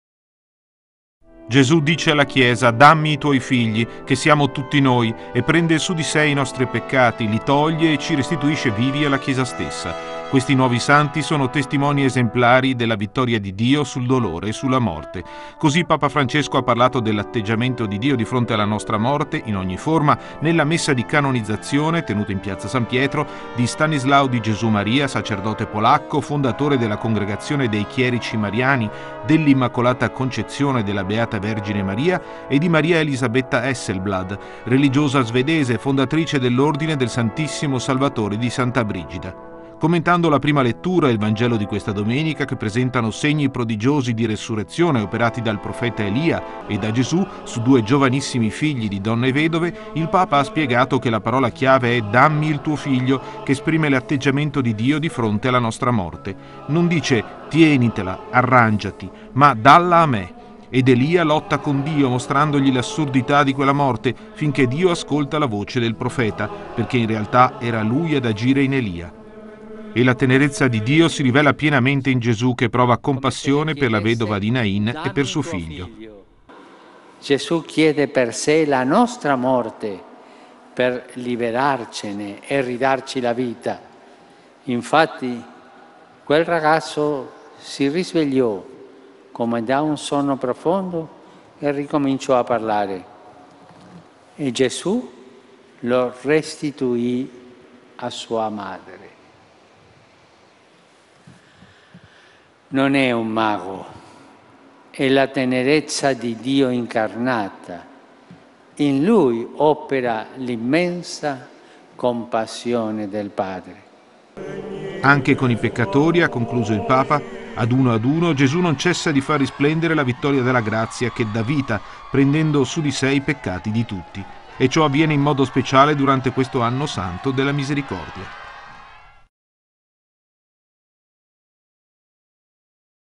Il Papa: Gesù si prende la nostra morte e ci ridà la vita
Gesù dice alla Chiesa: “dammi i tuoi figli” che siamo tutti noi, e prende su di sé i nostri peccati, li toglie e ci restituisce vivi alla Chiesa stessa. Questi nuovi santi sono testimoni esemplari di della vittoria di Dio sul dolore e sulla morte. Così Papa Francesco ha parlato dell’atteggiamento di Dio di fronte alla nostra morte, in ogni forma, nella messa di canonizzazione, tenuta in Piazza San Pietro, di Stanislao di Gesù Maria, sacerdote polacco, fondatore della congregazione dei Chierici mariani dell’Immacolata Concezione della Beata Vergine Maria e di Maria Elisabetta Hesselblad, religiosa svedese, fondatrice dell’ordine del Santissimo Salvatore di Santa Brigida.
Commentano la prima lettura e  ...